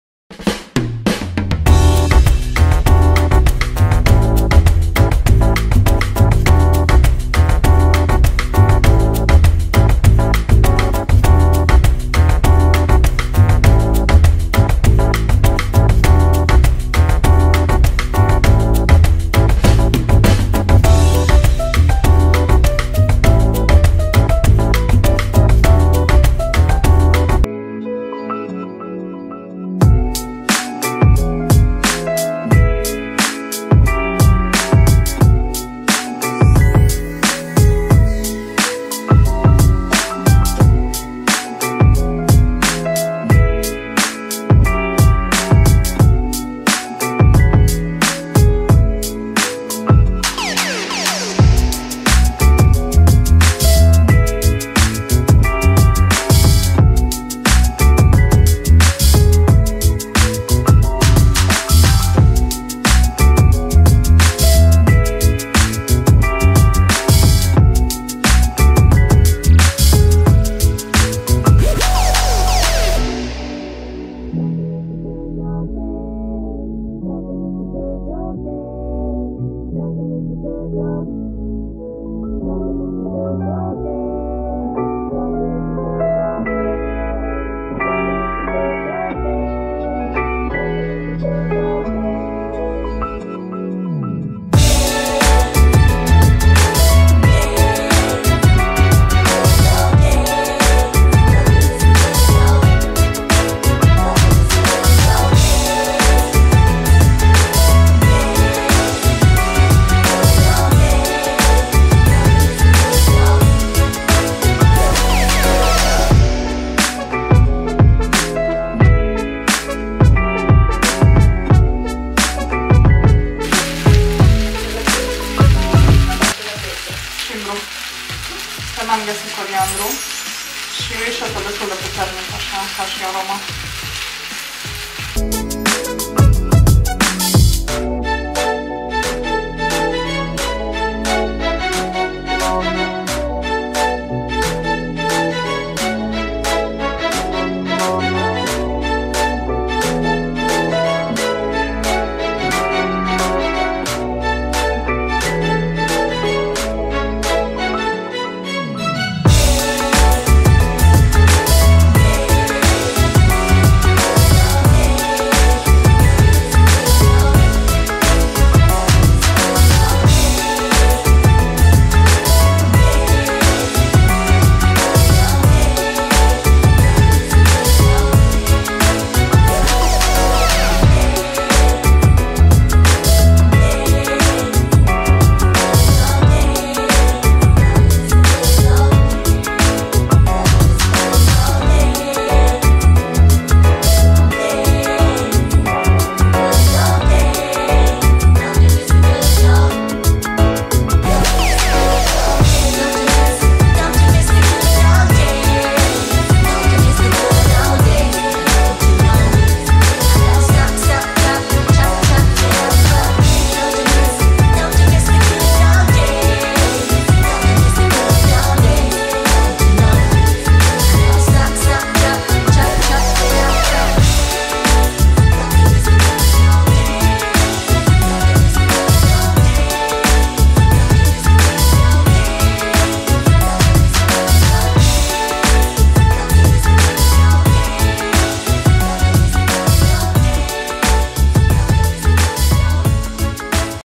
Curry vegan cu vinete și lapte vegetal de cocos, rețetă de amurg
Bună seara, o nouă miercuri de gătit!

În episodul de azi sora ne arata cum să facem un curry vegan, super delicios cu orez negru 🖤